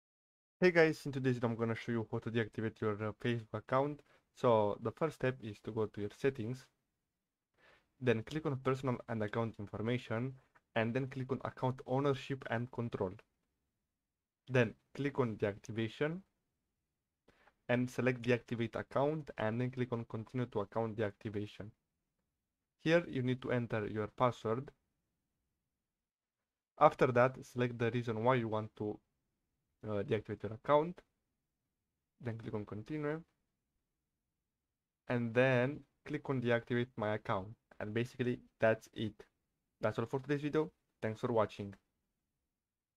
How to Deactivate Facebook Account 2024
In today's video I will show you how to deactivate facebook account.

0:00 Intro
0:05 How to Deactivate Facebook Account